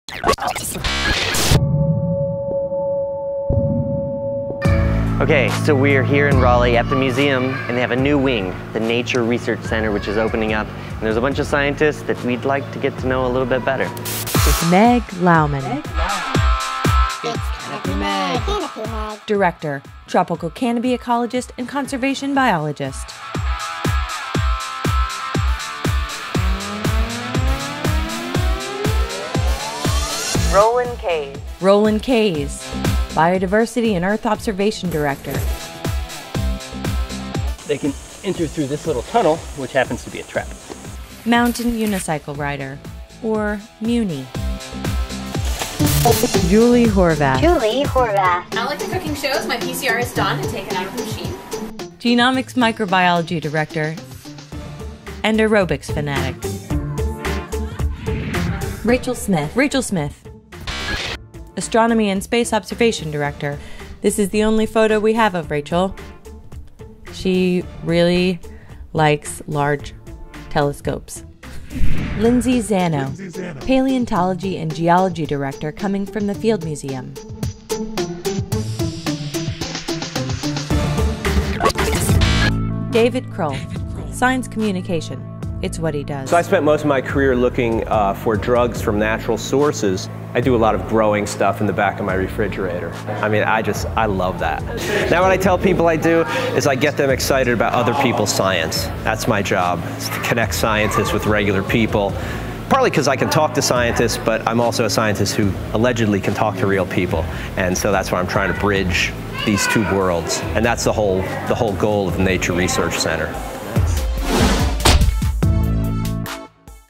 The New Directors of the Nature Research Center in Raleigh, NC
Meg Lowman, the director of the the Nature Research Center asked us to make a short video about some of the new directors. We of course, jumped at that chance. Meet Rachel Smith, Lindsay Zanno, Roland Kays, Julie Horvath and David Kroll.

Special thanks to Haley Chamberlain for the voice over work and Michelle Lotker and Jonas Stenstrom for their video expertise!

Edited and Produced by Rob Nelson